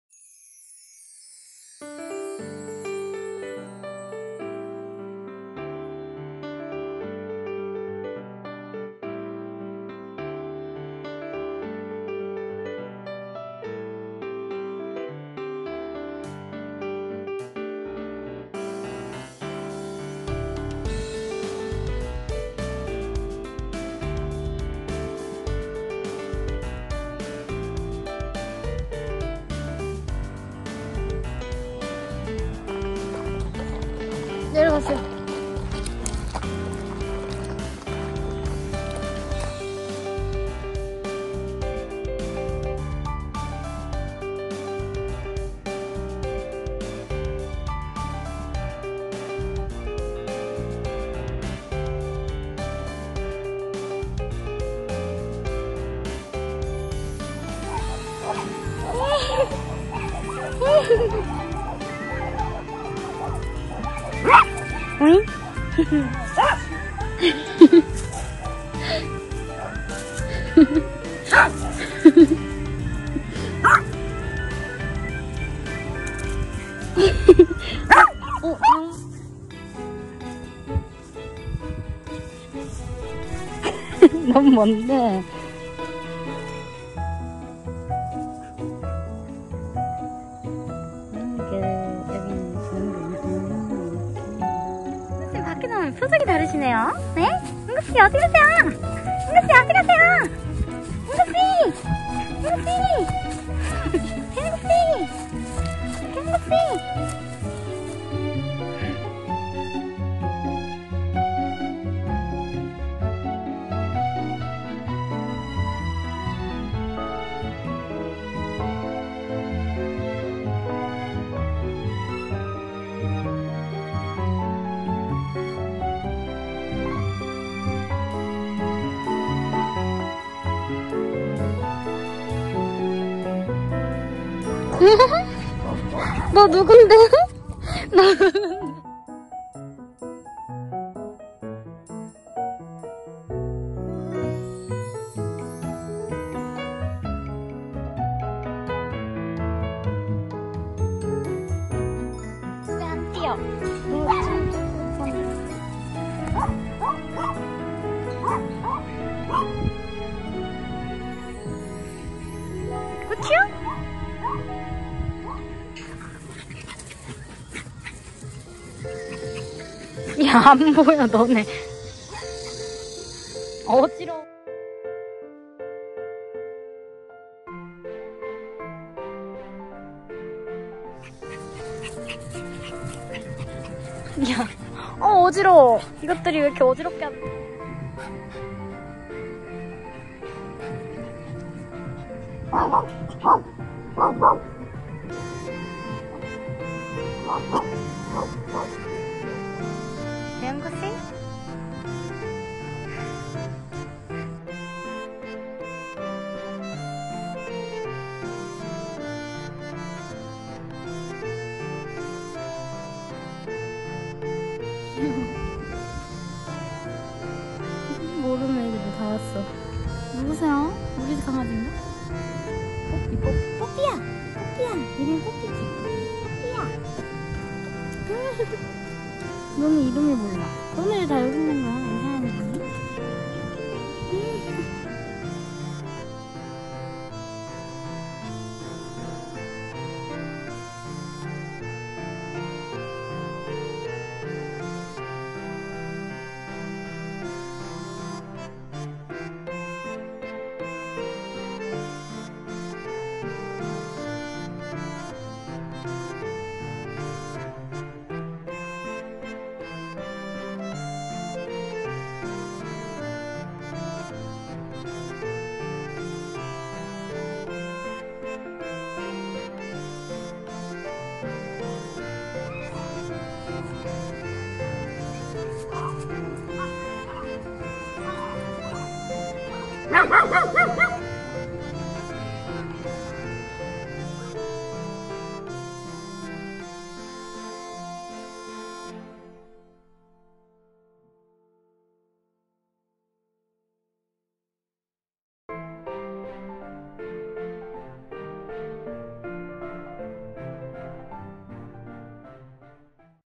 대전/계룡 강아지 운동장&애견카페 "퍼피스그린" 다녀온 후기
은구와 은솔이가 한달간 계룡에서 유배 생활을 했어요. 산좋고 물좋은 계룡에서 완전 이쁜 애견카페를 발견 !! 새로운 친구도 사귀고 신나게 뛰어놀다 왔어요!!

1. 첫째(First): 은지 "EUNJI" (Poodle)
    성별: 여아 (Famale)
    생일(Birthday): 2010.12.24
    특징(Character): 걷는 걸 싫어하고 안겨있는걸 너무 좋아하는 할머니, 먹을꺼 앞에서는 주인이고 없음. 요즘 동안 미모 뿜뿜 (Eunji hates walking and loves to eat. Eunji is 10 years old. But these days my face is getting younger.)

2. 둘째(Second): 은구 "EUNGU" (Maltese)
    성별: 남아 (Male)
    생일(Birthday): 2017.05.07
    특징(Character): 부정교합이라 메롱 하면서 자는 우리막내. 오합지졸이지만 모아놓으면 귀여운 상남자. 잘난거라곤 목소리큰거.(He open his mouth because of malocclusion. Eyes, nose and mouth are cute guys. The good point is a big voice. My youngest child.)